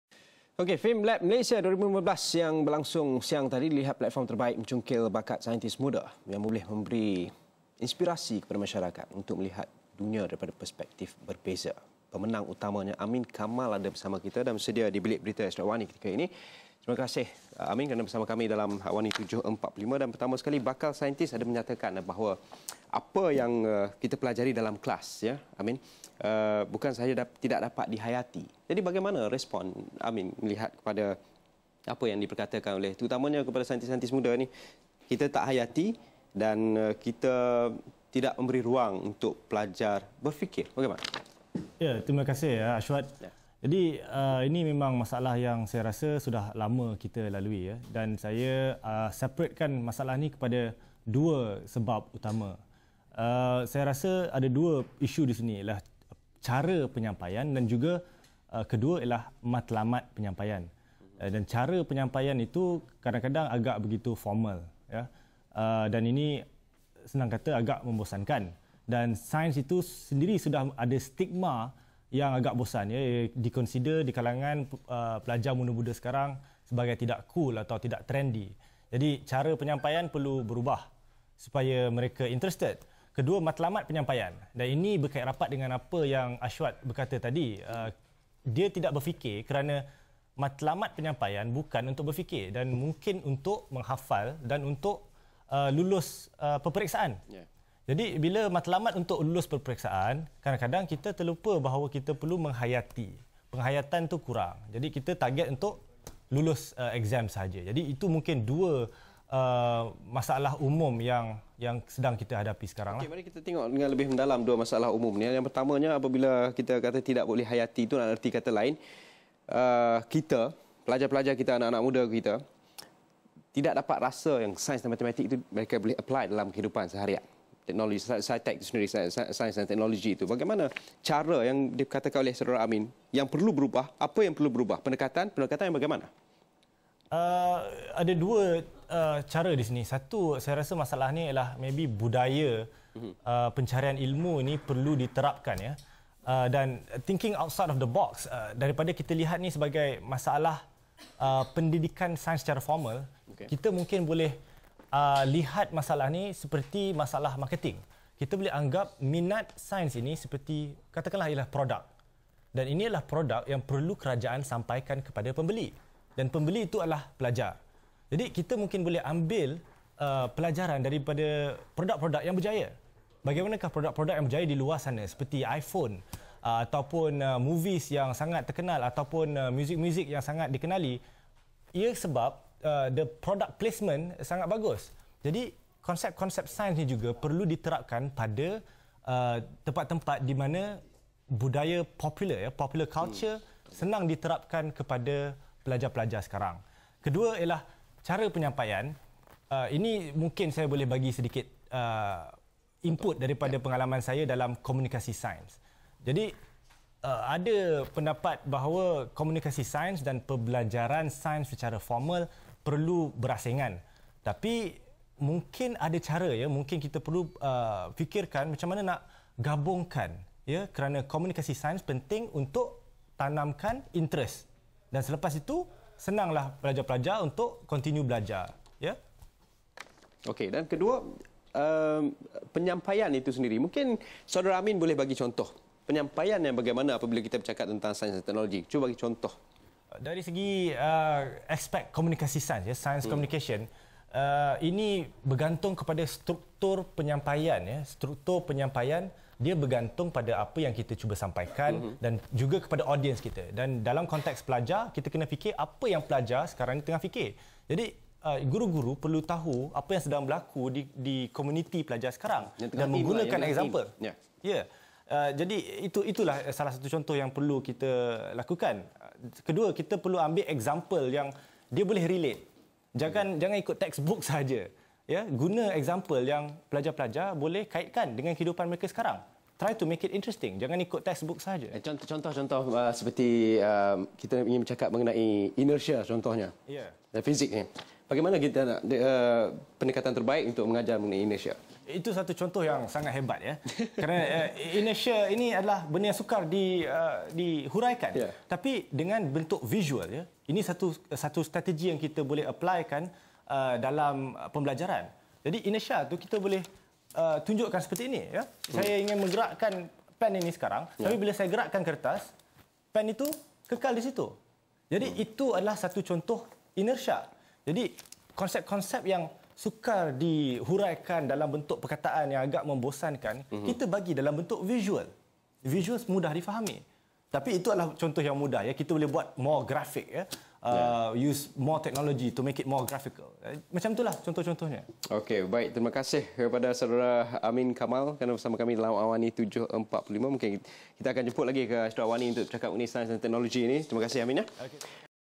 Famelab platform terbaik cungkil bakat saintis muda
Famelab Malaysia 2015 yang berlangsung siang tadi dilihat platform terbaik mencungkil bakat saintis muda yang boleh memberi inspirasi kepada masyarakat untuk melihat dunia daripada perspektif berbeza. Pemenang utamanya, Ameen Kamal ada bersama kita.